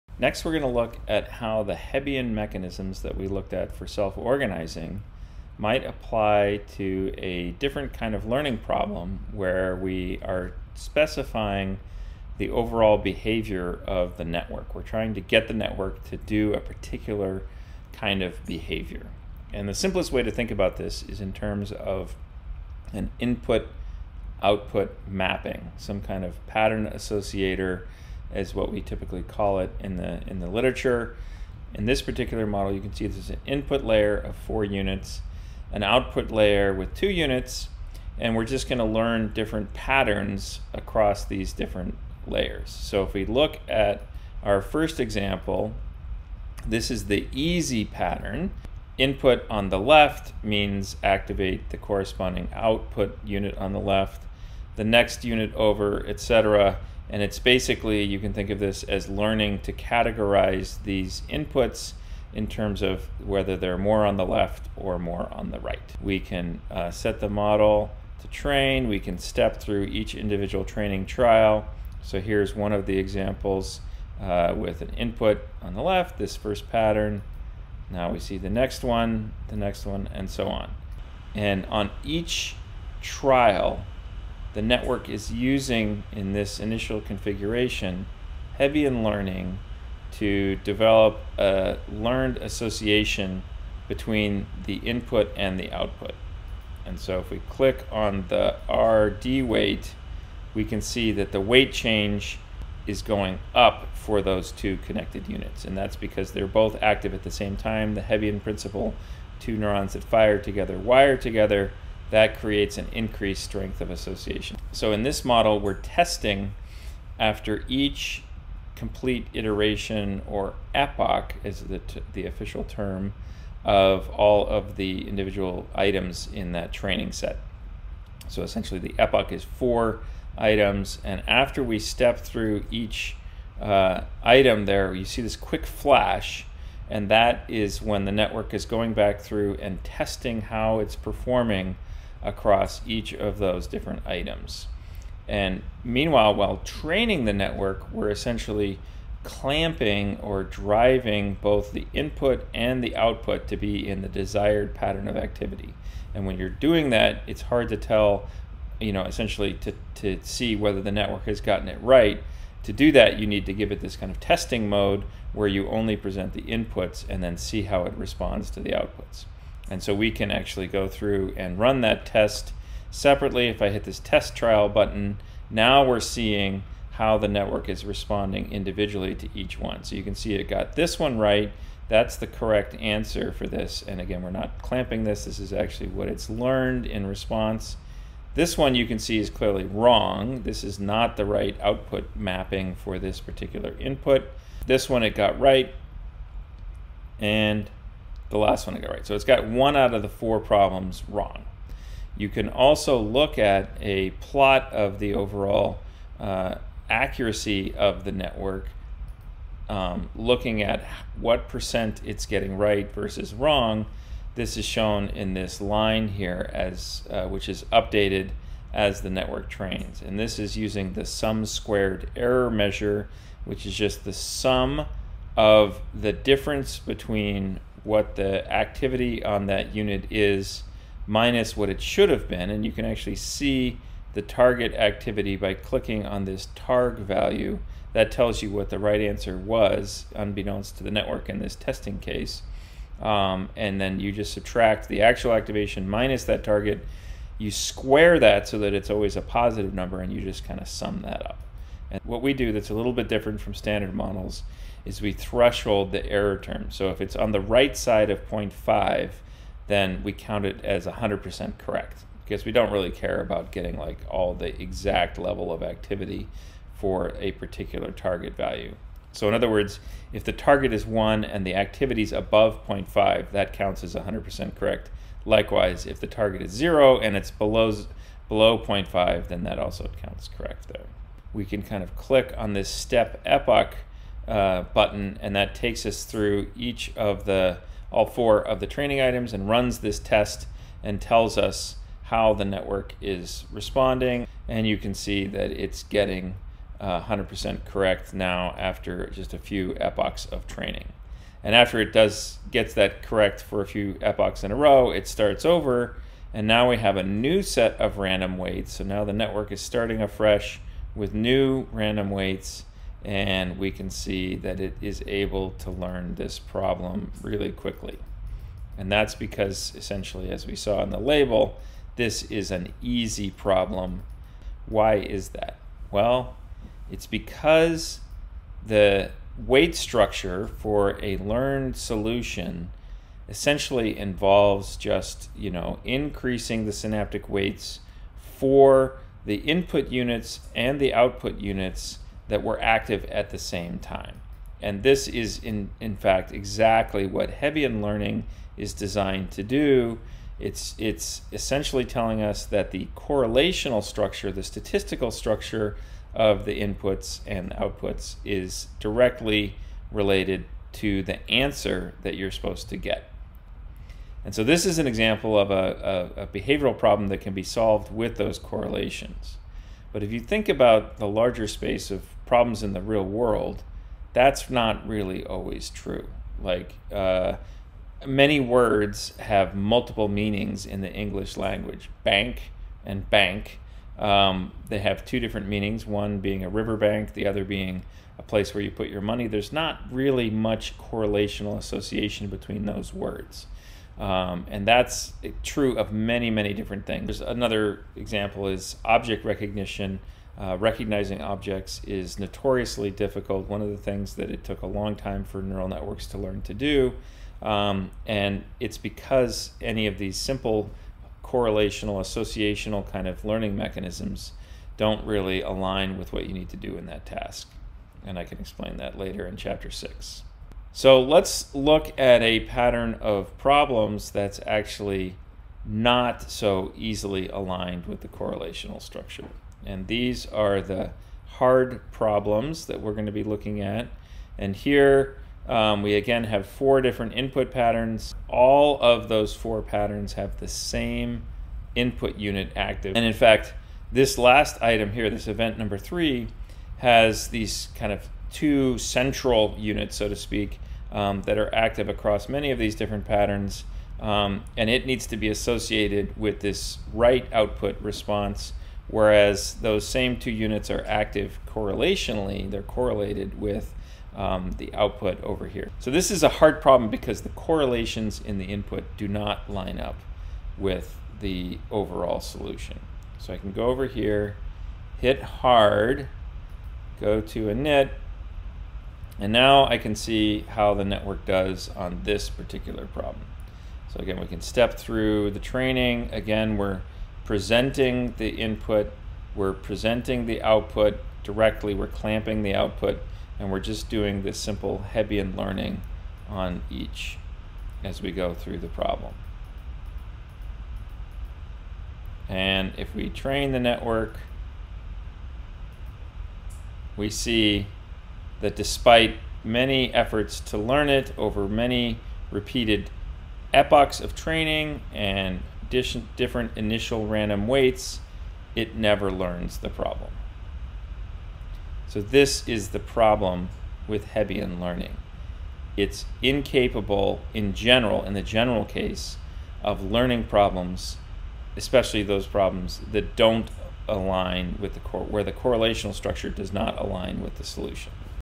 CCN Course 2020, Learning 5: Pattern Associator
Lecture 5 of 9 for Chapter 4: Learning

The limits of Hebbian learning are revealed in tasks that associate input patterns with output patterns.